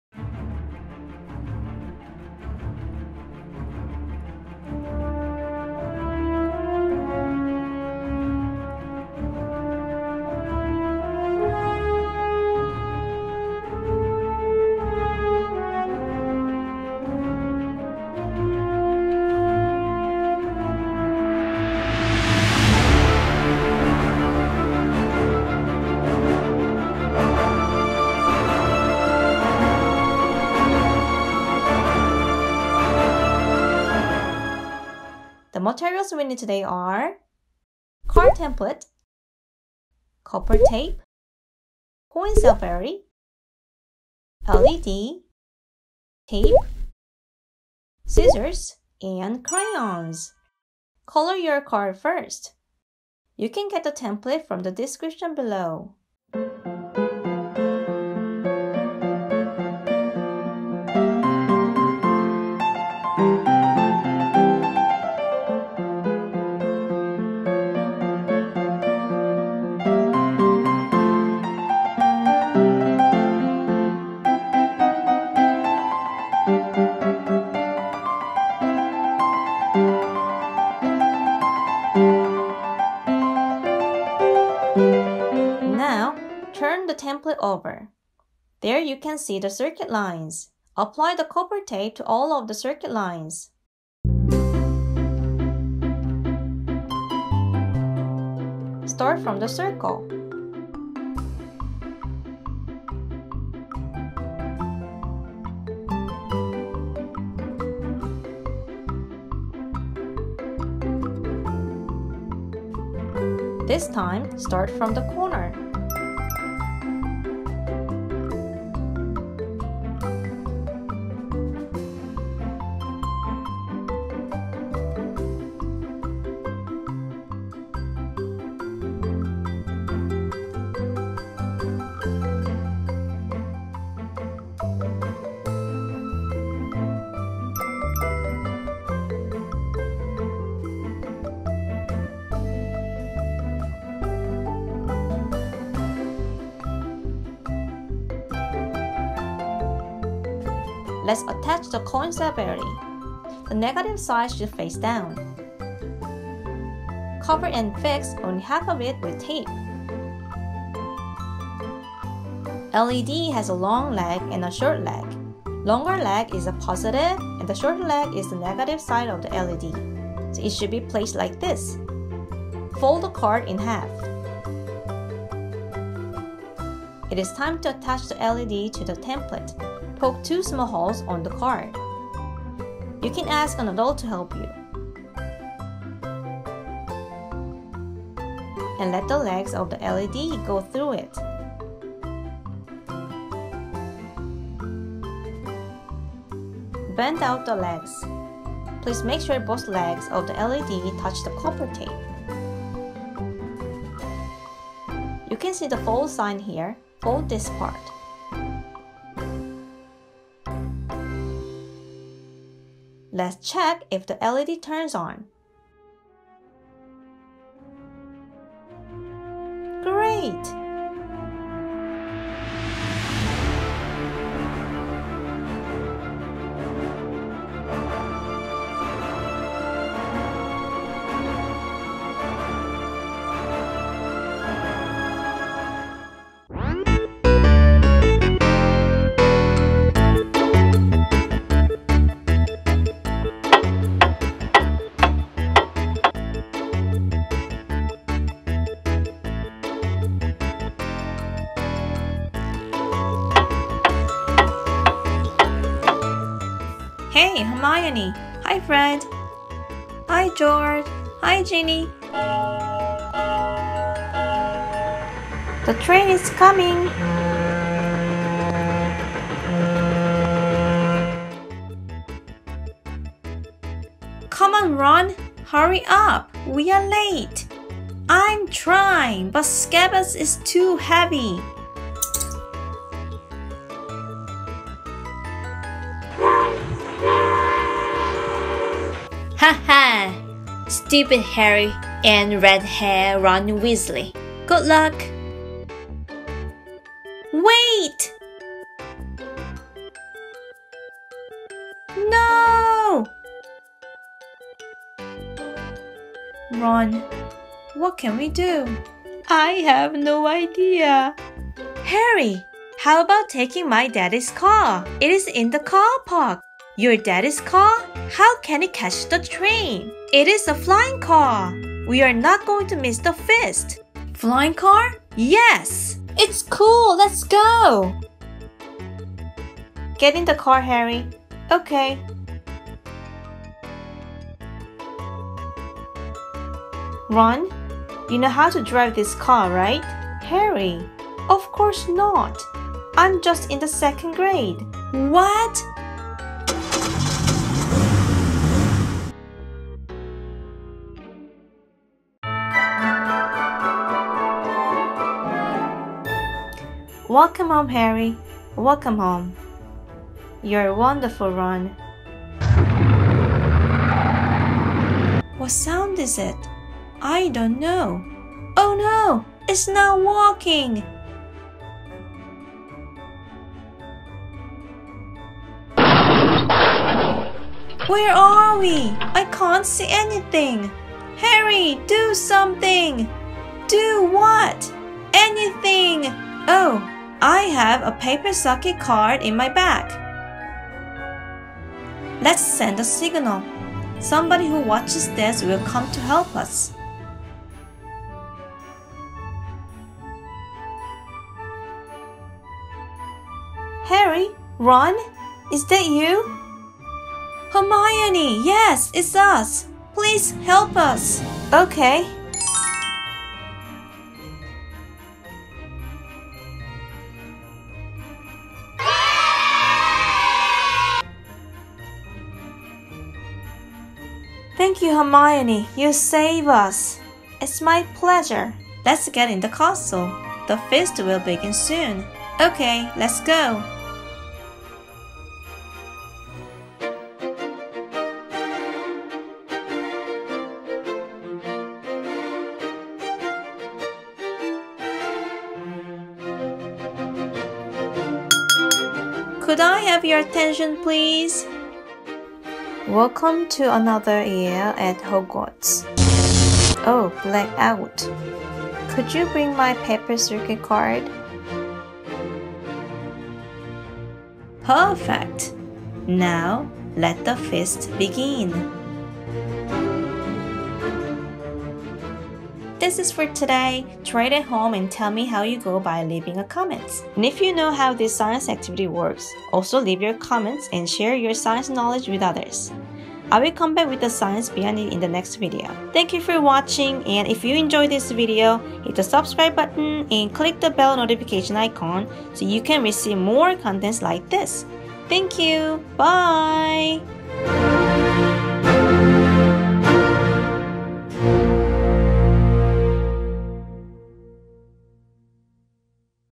Harry Potter Paper Circuit Card
Today we are going to make [Harry Potter paper circuit card]. Look at the magic wand and the bright light at the tip of the wand. Amazing, right? Follow me and you can make your own magic wand. Let’s get to it.

Use the link below for Harry Potter card template